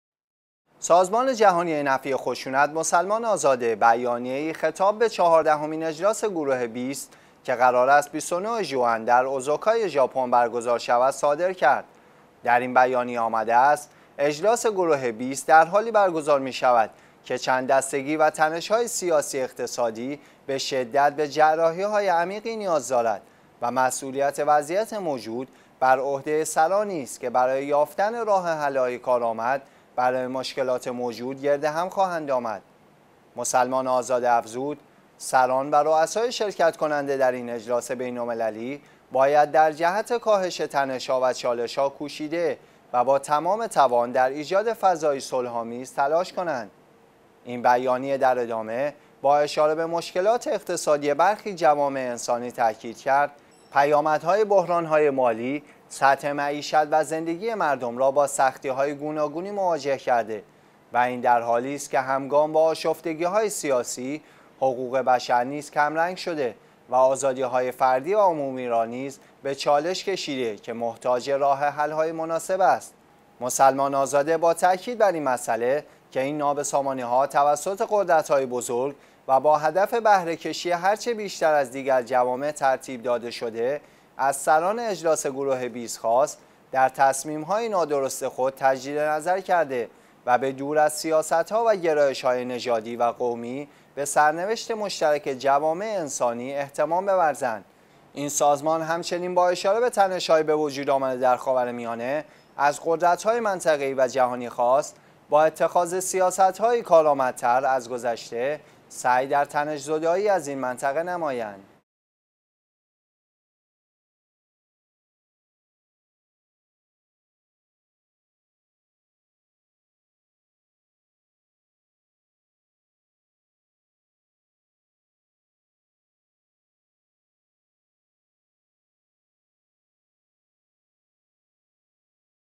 مسلمان آزاده خطاب به اجلاس بیست: آتش بحران های بین المللی را خاموش کنید
سازمان جهانی نفی خشونت ـ مسلمان آزاده ـ بیانیه ای خطاب به چهاردهمین اجلاس گروه بیست که قرار است 29 ژوئن در اوزاکای ژاپن برگزار شود، صادر کرد.
در این بیانیه آمده است: اجلاس گروه بیست در حالی برگزار می شود که چند دستگی و تنش های سیاسی - اقتصادی به شدت به جراحی های عمیقی نیاز دارد و مسئولیت وضعیت موجود بر عهده سرانی است که برای یافتن راه حل های کارآمد برای مشکلات موجود، گرد هم خواهند آمد.
مسلمان آزاده افزود: سران و روسای شرکت کننده در این اجلاس بین المللی باید در جهت کاهش تنش ها و چالش ها کوشیده و با تمام توان در ایجاد فضایی صلح آمیز تلاش کنند.
این بیانیه در ادامه با اشاره به مشکلات اقتصادی برخی جوامع انسانی تاکید کرد: پیامدهای بحران های  مالی، سطح معیشت و زندگی مردم را با سختی های گوناگونی مواجه کرده و این در حالی است که همگام با آشفتگی های سیاسی، حقوق بشر نیز کمرنگ شده و آزادی های فردی و عمومی را نیز به چالش کشیده که محتاج راه حل هایی مناسب است.
مسلمان آزاده با تاکید بر این مسئله که این نابسامانی ها توسط قدرت های بزرگ و با هدف بهره کشی هرچه بیشتر از دیگر جوامع ترتیب داده شده، از سران اجلاس گروه بیست خواست در تصمیم های نادرست خود تجدیدنظر کرده و به دور از سیاست ها و گرایش های نژادی و قومی به سرنوشت مشترک جوامع انسانی اهتمام بورزند
این سازمان همچنین با اشاره به تنش های بوجود آمده در خاورمیانه از قدرت های منطقه ای و جهانی خواست با اتخاذ سیاست هایی کارآمدتر از گذشته، سعی در تنش زدایی از این منطقه نمایند.